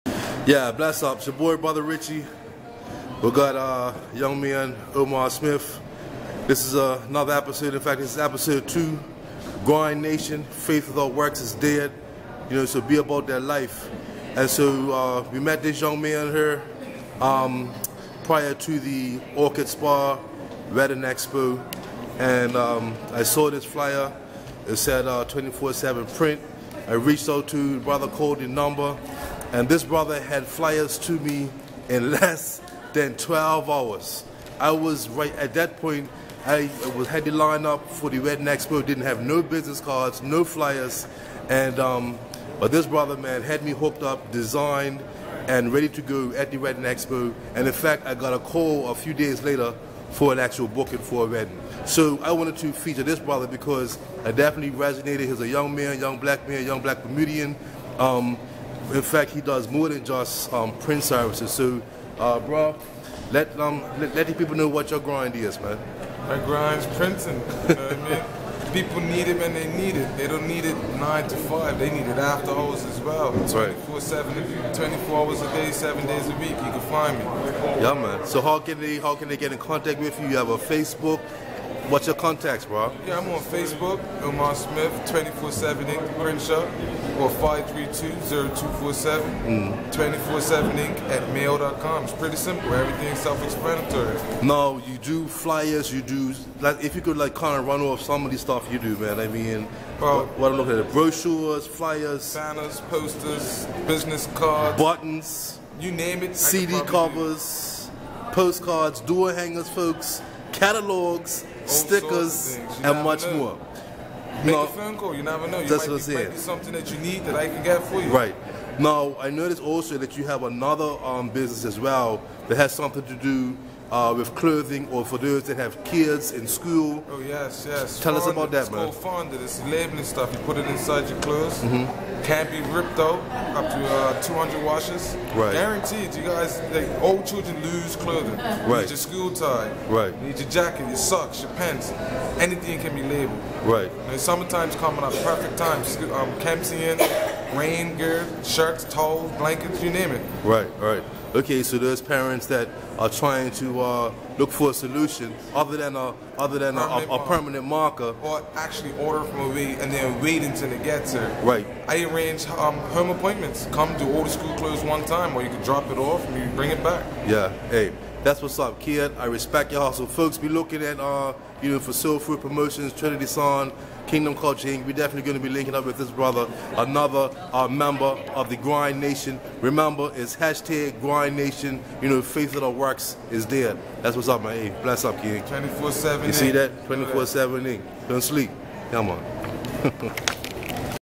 GRINDNATION Episode 2 feat. Omar Smith of 24/7 The Print Shop
GRINDNATION - "Faith without works is DEAD!" Be about dat LIFE! Omar Smith is a young entrepreneur that defines GRIND to the utmost. He lives up to his business' name and is always ready to meet his clients need on time and with quality work!